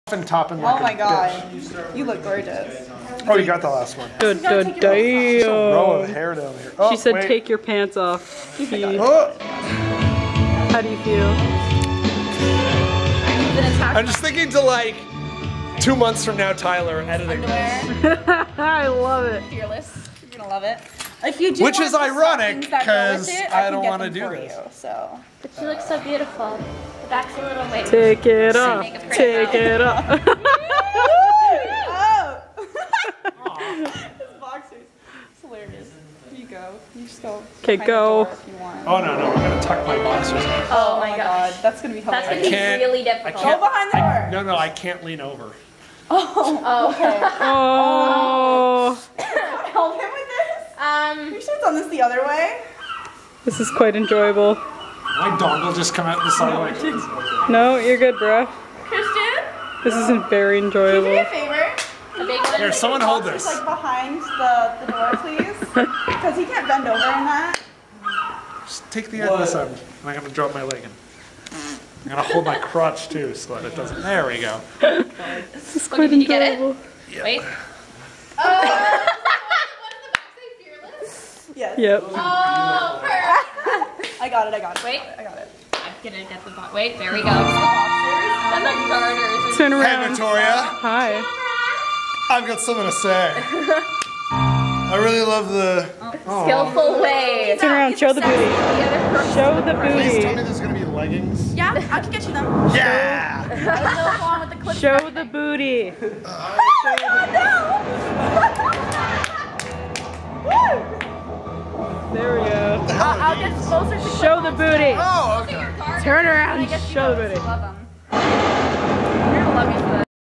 Behind The Show | Tyler Tries On His Costume | Riku Horror Fandom Show
Tyler tries on his costumes for Axel / Frank for our parody musical of Rocky Horror and Kingdom Hearts, The Riku Horror Fandom Show